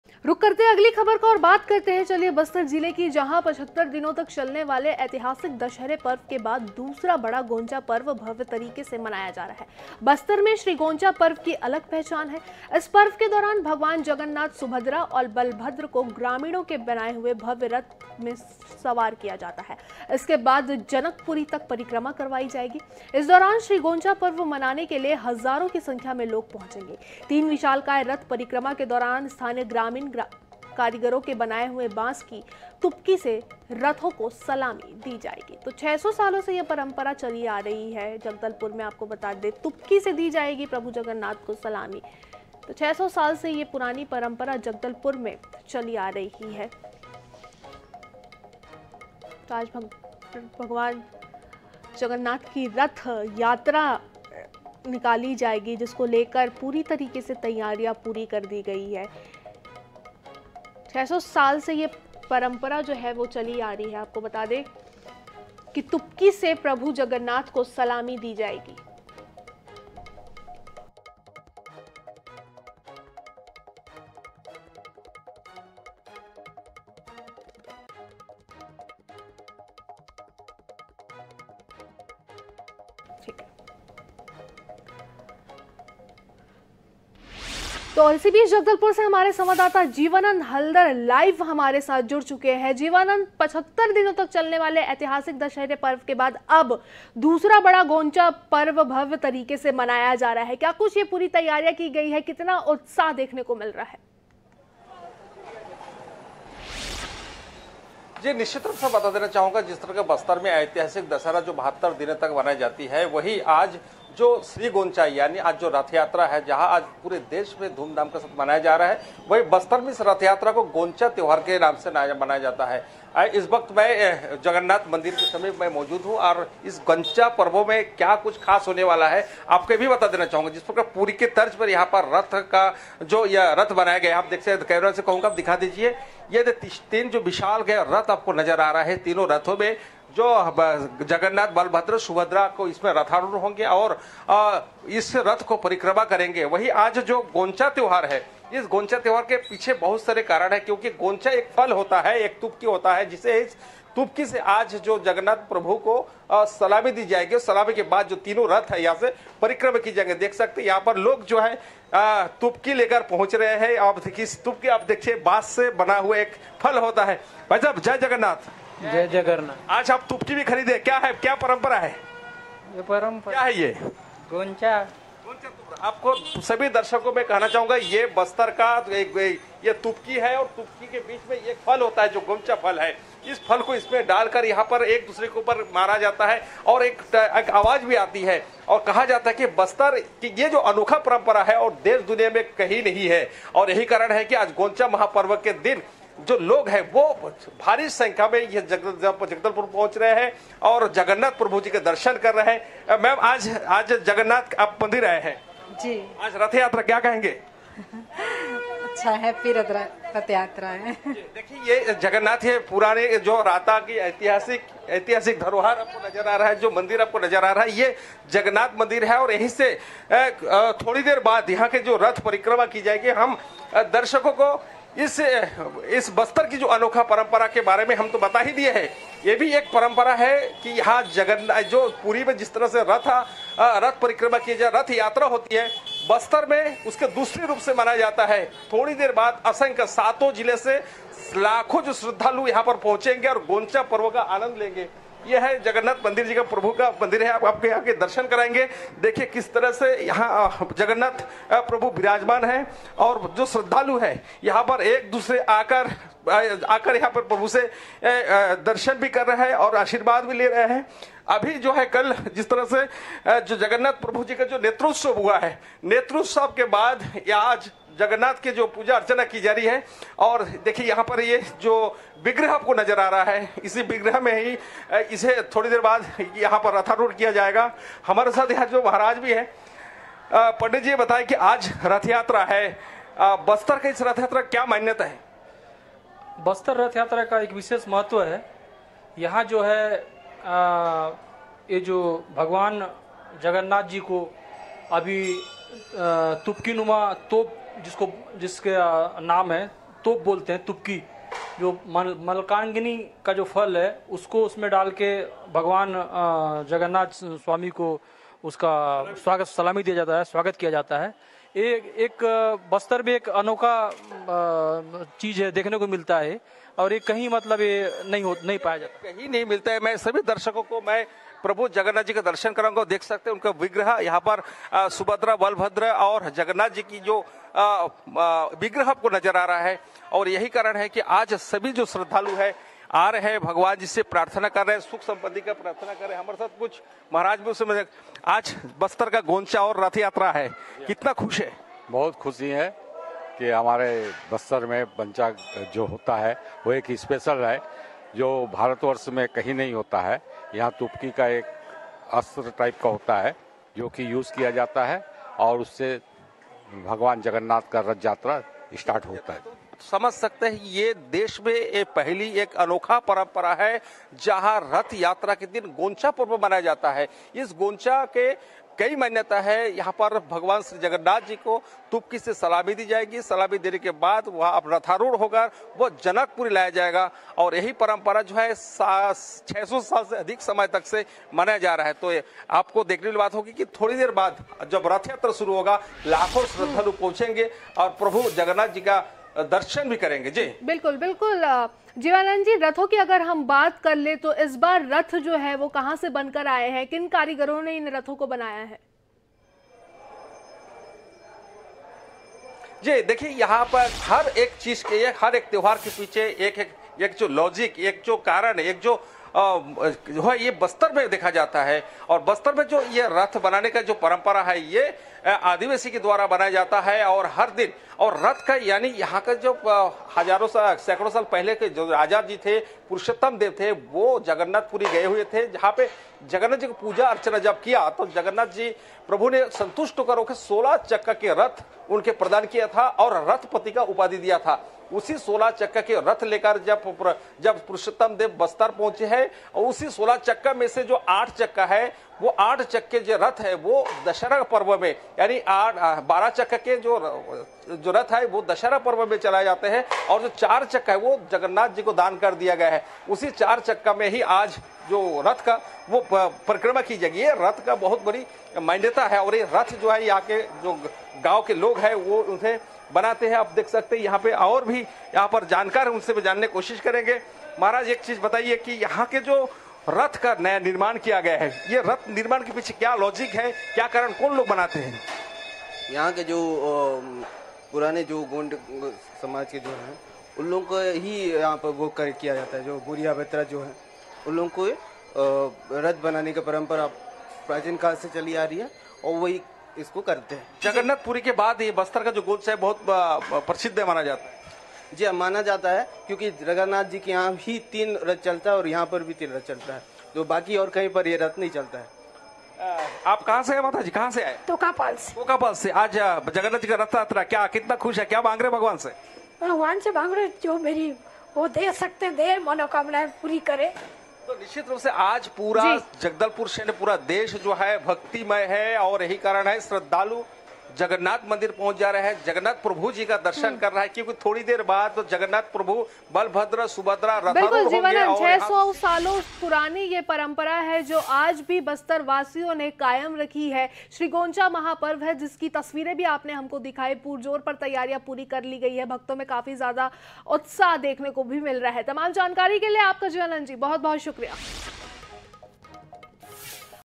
गोंचा महापर्व की जश्न में दुबे लोग, तुपकी से जगन्नाथ महाप्रभु को दी जाएगी सलामी | Jagdalpur News
INH,  गोंचा महापर्व की जश्न में दुबे लोग, तुपकी से जगन्नाथ महाप्रभु को दी जाएगी सलामी | Jagdalpur News

Incoming Search Terms: